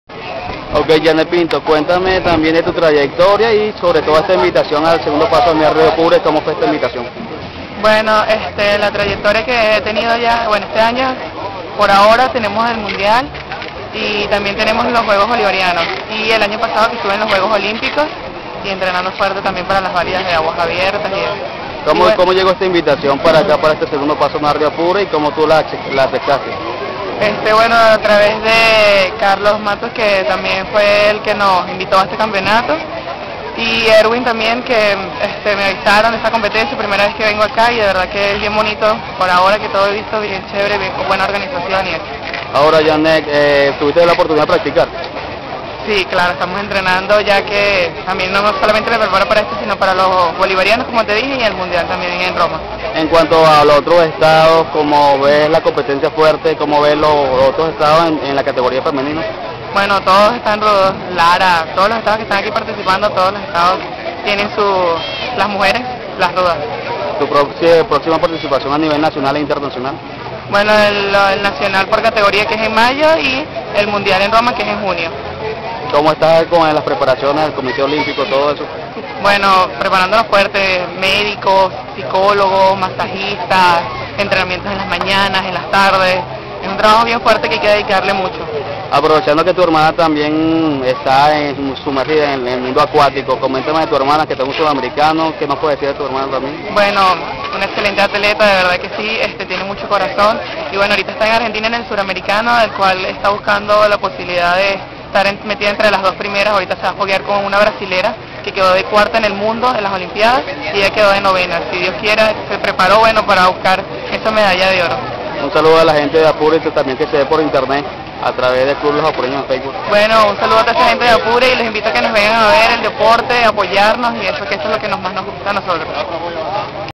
Entrevista con Yanel Pinto Campeona Absoluta Femenina del 2do. Paso a Nado al Rio Apure 2009.
Por su parte, la ganadora del 2do. Paso a Nado al Río Apure absoluta femenino en representación de Miranda Yanel Pinto dijo que se encuentra actualmente en la preparación para los Juegos Bolivarianos y el Mundial  que se va a efectuar en Roma en el mes de julio del presente año. En lo relacionado al desafío al Río Apure comentó que no es fácil sobre todo nadar contra la corriente y que le gusto la trayectoria de la ruta y argumento Pinto que piensa repitar para la próxima edición del paso a nado al Rio Apure. Video por Eduardo Galindo Peña.